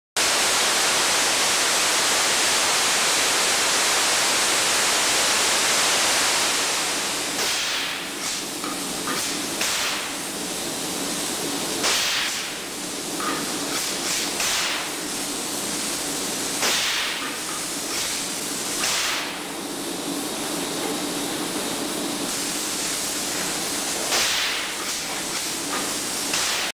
Epoxy Pipe Rehabilitation
See our approved applicators use a combination of spin casting and spray applications to rehabilitate a deteriorating pipe.